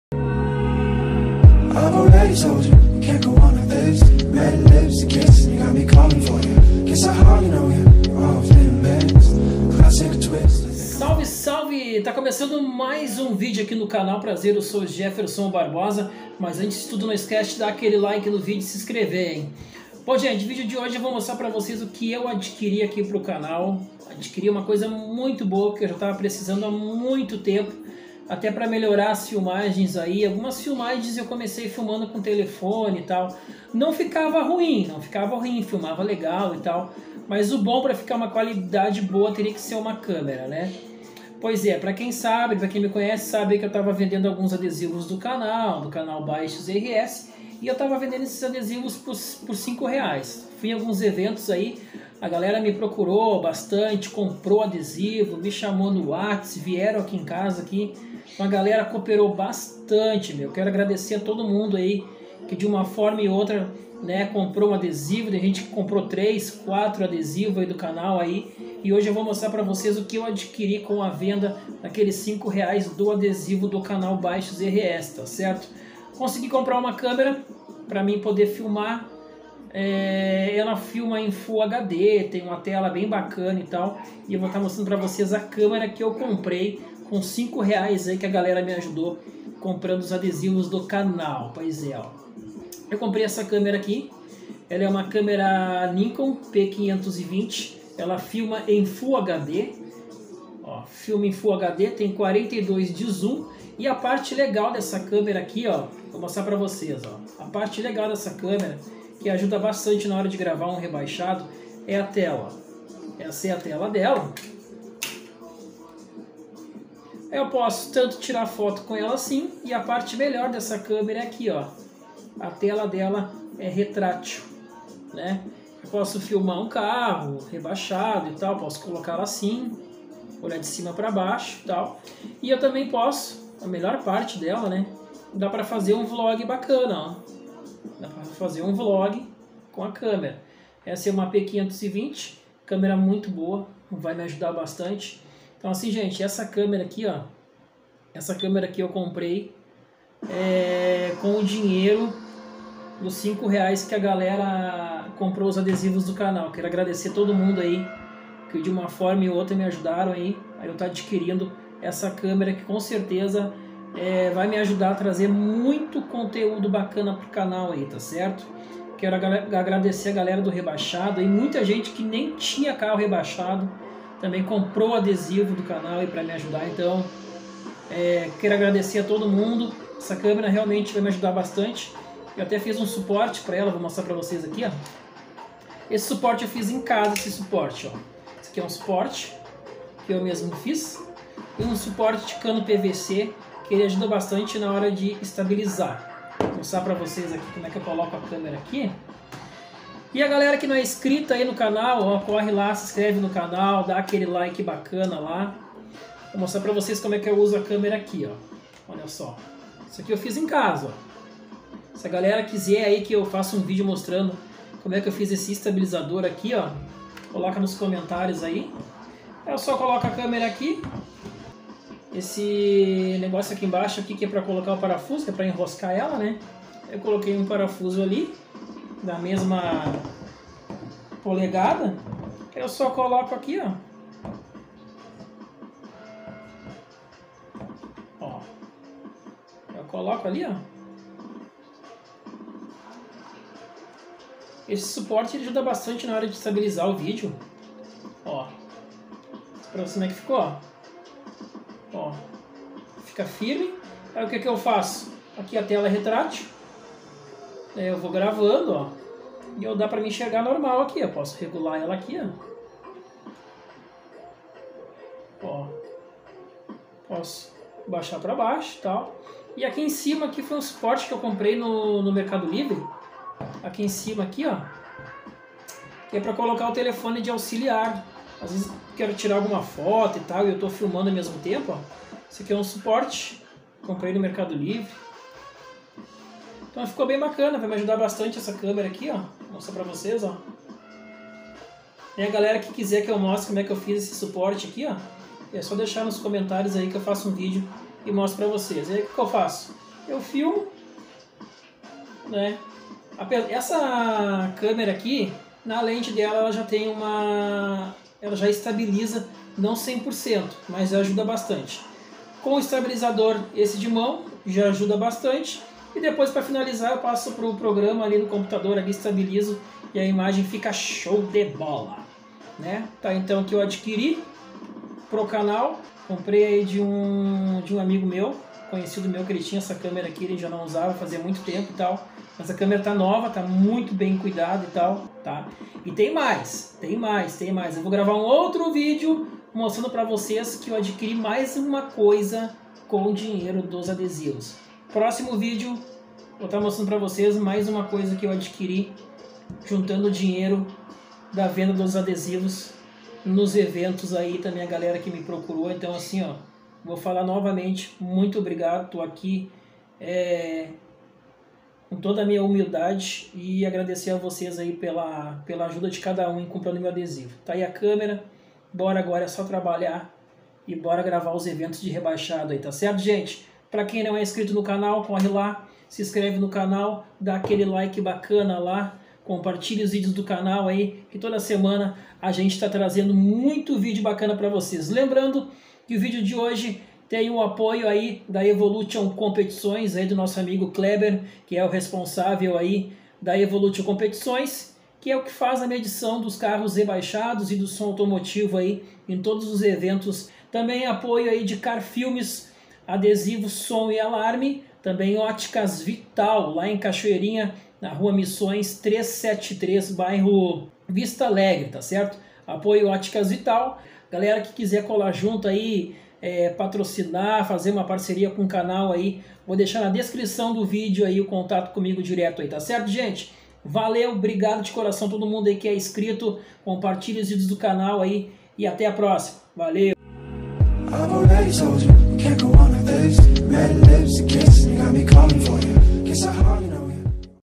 Camera Nova do Canal
Hoje vou mostrar para todos a camera que eu comprei para o canal, gostaria de agradecer a todos de coração que de uma forma ou outra me ajudaram comprando o adesivo do canal, com esse dinheiro consegui comprar uma câmera para fazer os vídeos do canal. uma Nikon P 520 que vai me ajudar muito mas gravaçoēs. Obrigado a todos🙏🙏

📲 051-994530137 Esse é o contato para parcerias comercial ou você que queira gravar um vídeo do sei carro ou contratar para even